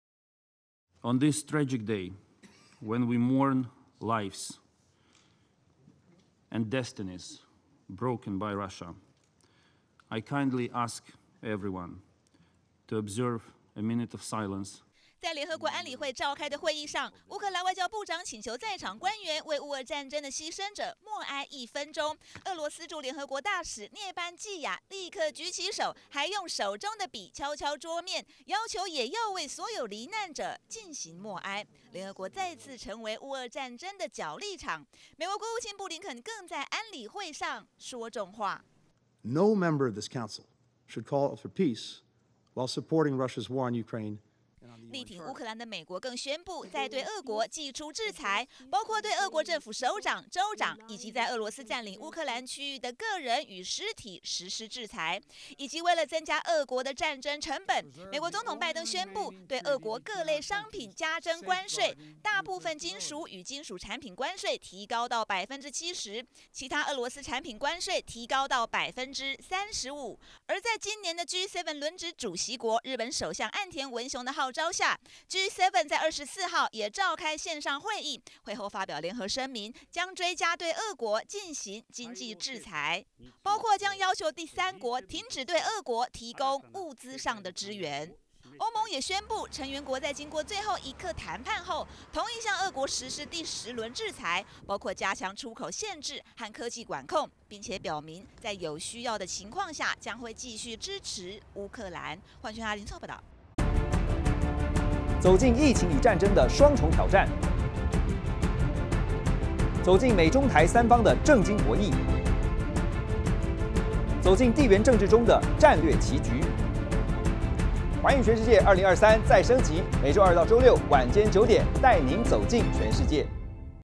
烏俄戰爭滿周年 美歐G7加強制裁俄羅斯｜寰宇新聞
在俄羅斯入侵烏克蘭滿一周年之際，白宮再度宣布，對俄羅斯祭出制裁。G7國家也召開視訊會議，決定加強制裁俄國。而歐盟也通過，要向俄國實施第十輪制裁，包括加強出口限制。至於在聯合國安理會上，則是上演烏俄交鋒。